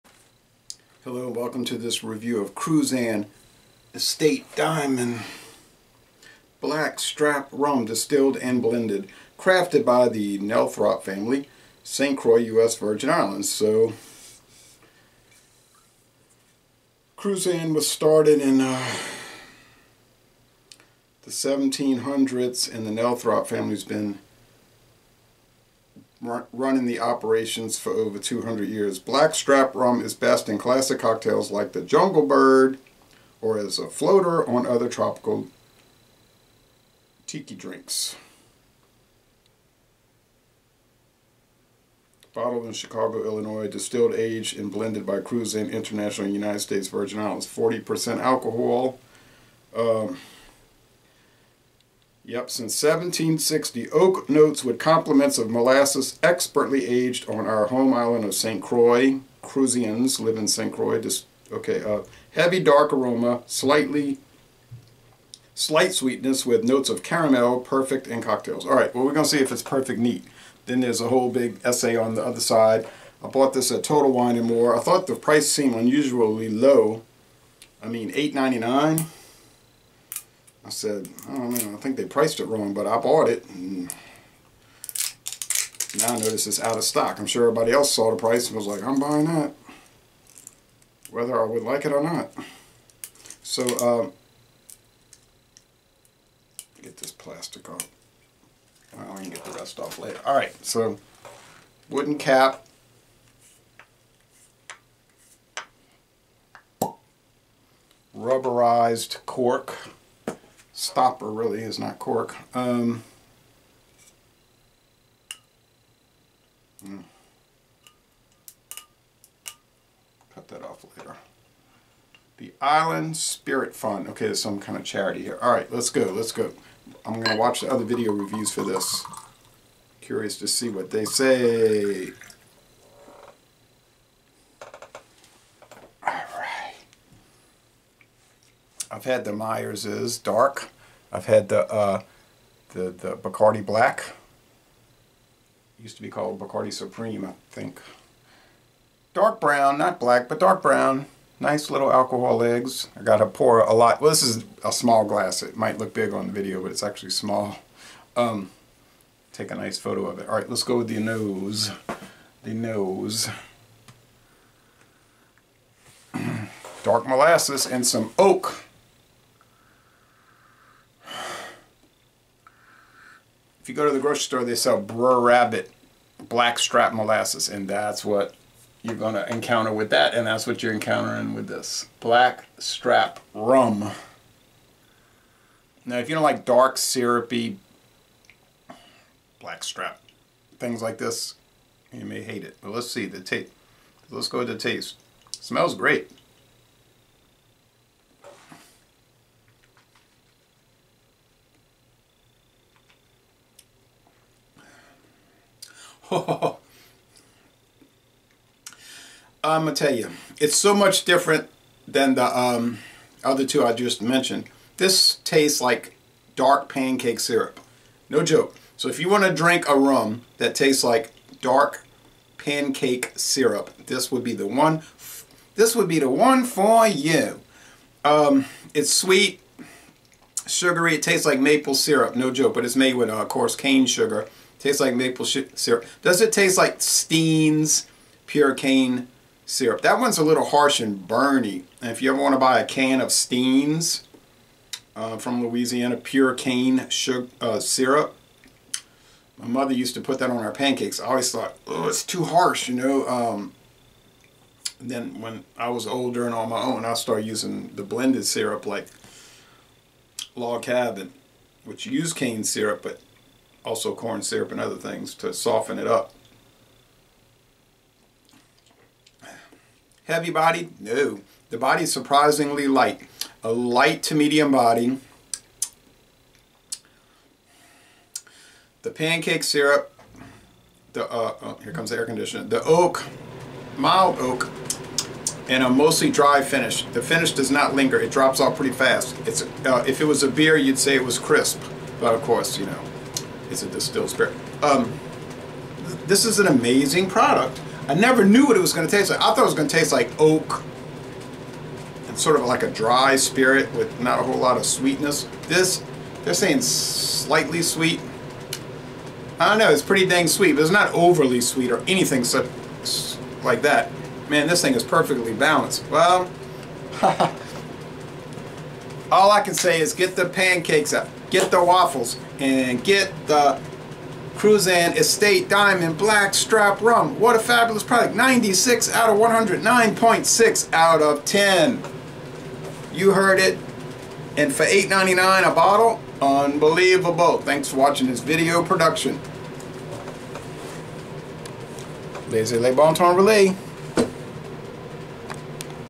Cruzan Estate Diamond Black Strap Rum
40% alcohol. Produced in the U.S. Virgin Islands. Bottled in Illinois. A Cruzan International brand. Company founded in 1760.

"A mixologist favorite, Cruzan® Black Strap Rum features robust licorice and molasses flavors that add an exotic element to your tropical favorites. Its most popular pairings include flavors such as lemon, lime, ginger and pineapple."